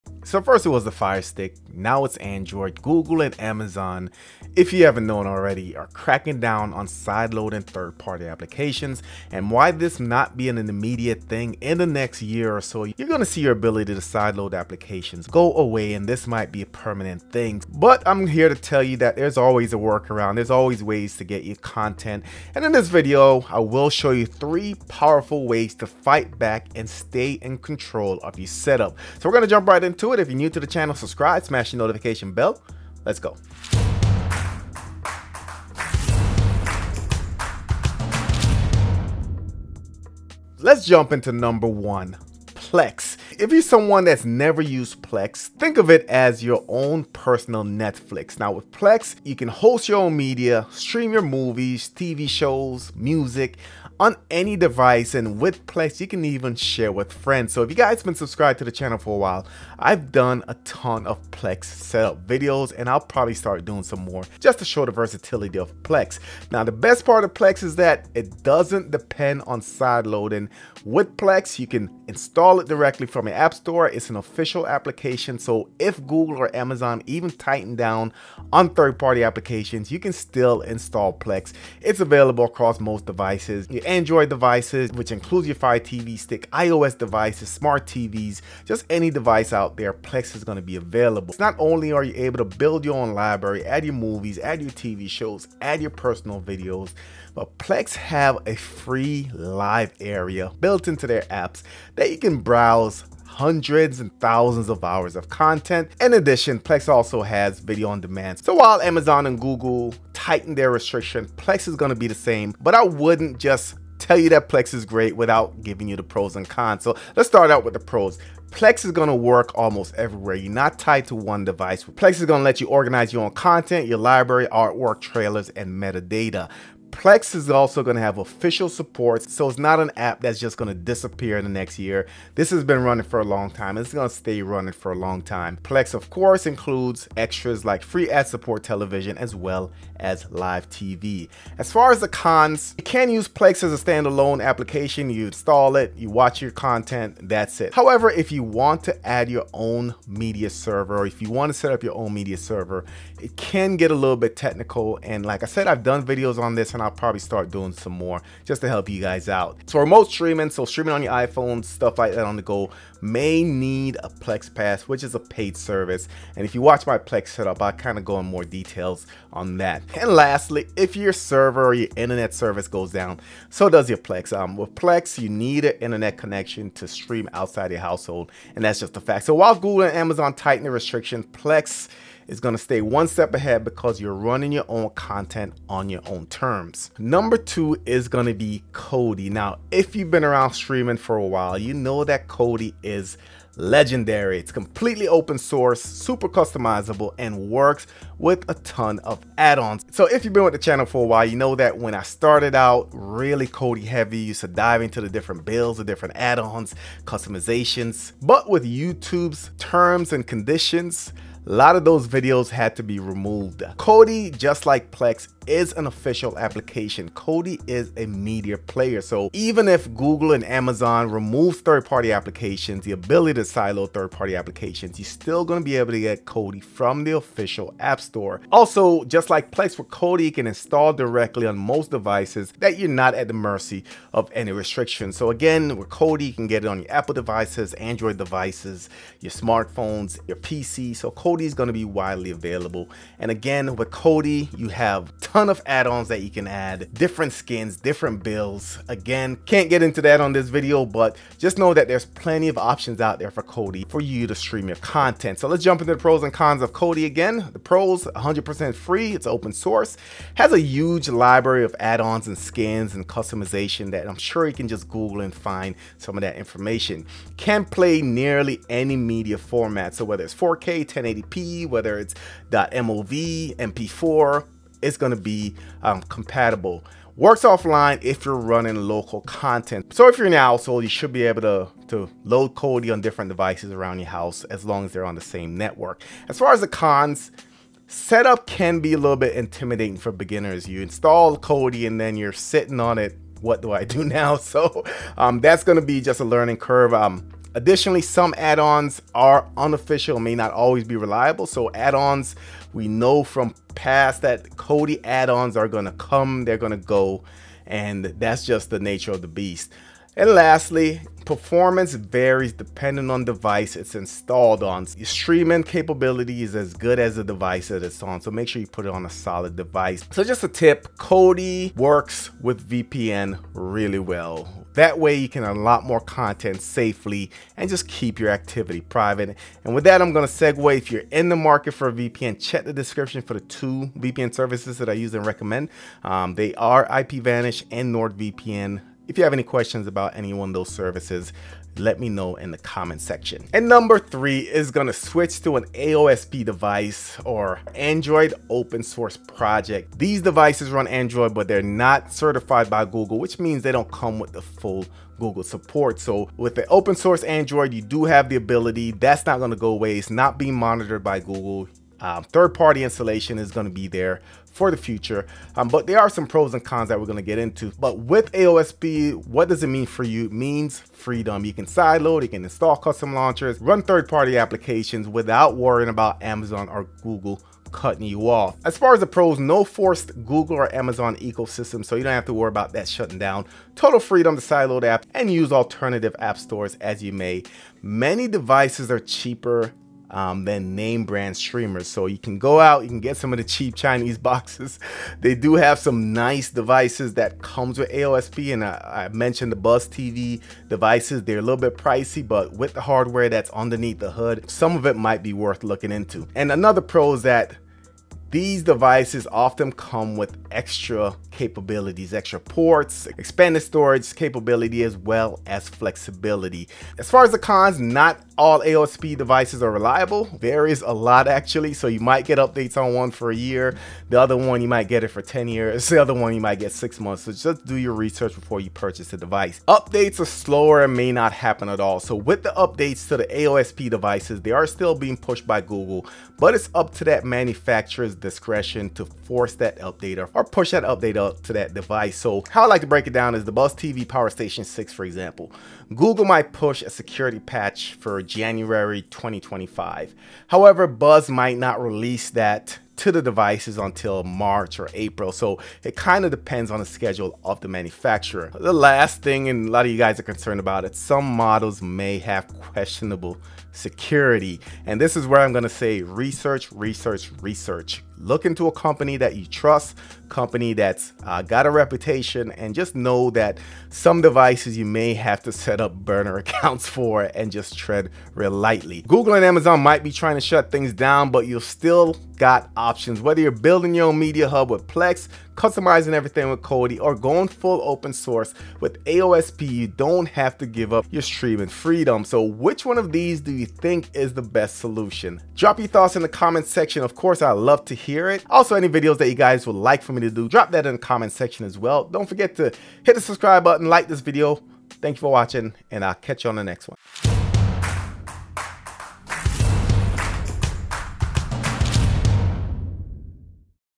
Amazon & Google BLOCK Sideloading! 3 Ways You Can Fight Back
First Firestick, now Android. With new restrictions making it harder to sideload apps, many are asking: how do we stop Google and Amazon from blocking apps? In this video, I’ll break down 3 powerful alternatives you can use right now:
✅ Plex – Turn your media library into your own streaming service.
✅ Kodi – Unlock customization, add-ons, and endless streaming options.
✅ AOSP Devices – Go full Android Open Source Project for total freedom.

I’ll explain the pros and cons of each option, show how they work on Android TV and Firestick, and help you decide the best way to keep control of your streaming setup.
⏱ Timestamps
0:00 - Intro
0:40 – Option 1: Plex
1:57 – Pros & Cons of Plex
3:07 – Option 2: Kodi
4:15– Pros & Cons of Kodi
5:44 – Option 3: AOSP Devices
6:19 – Pros & Cons of AOSP Devices
7:59 – What Are Your Thoughts?
💬 Which option do YOU think is the best solution? Plex, Kodi, or AOSP?